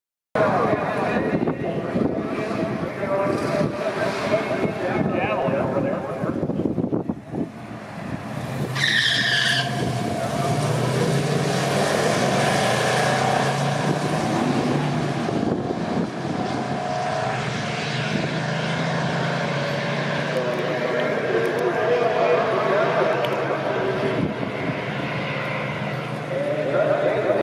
AMC RACES 2018-Cordova,IL-Rd 2-Street-1973 Javelin vs 1970 Rebel SST
Video curtesy of BH